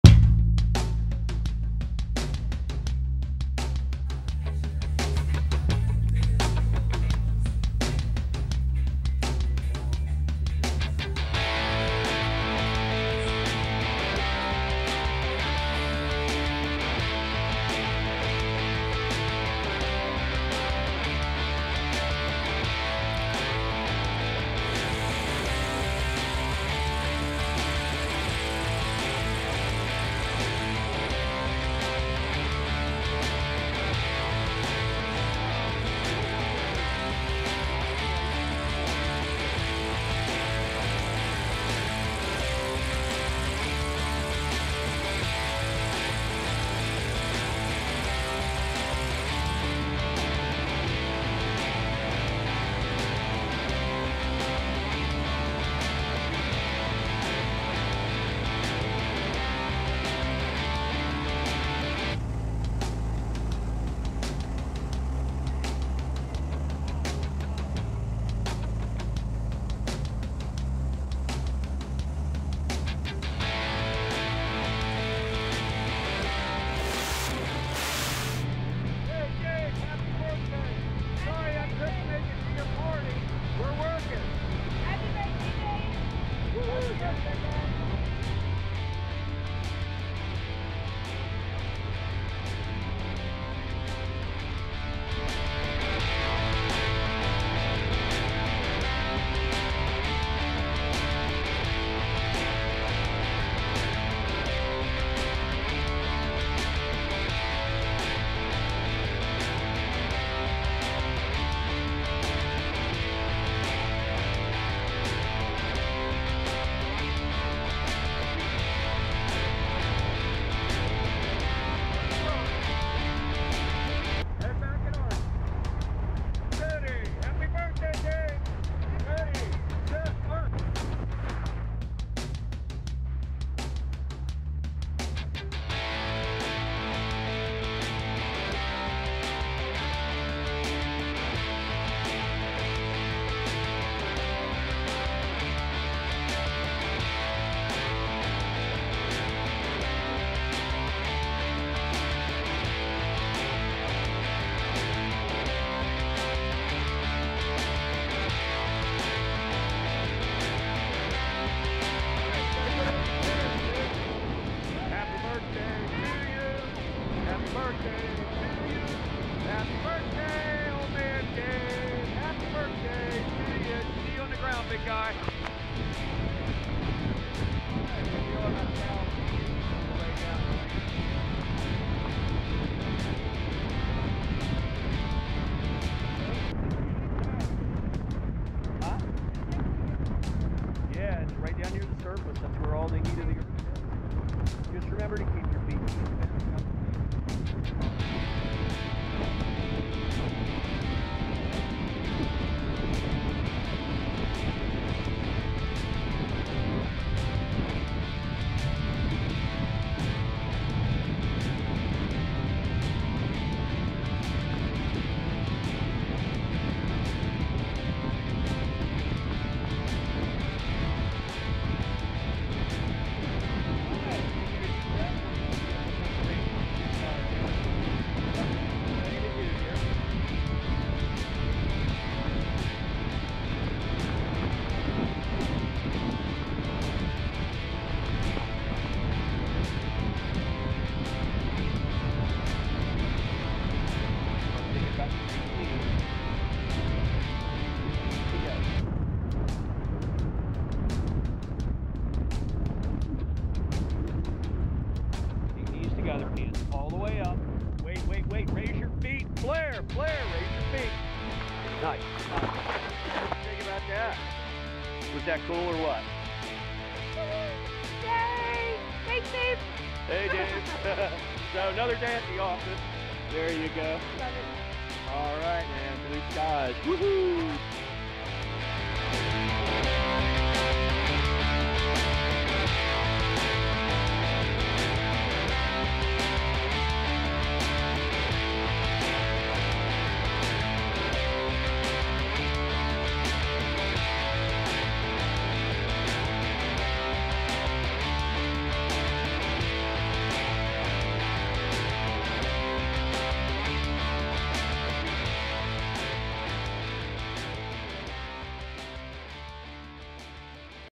Daves 70th Birthday
David Bodah's 70th Birthday wishes from J.R, Sides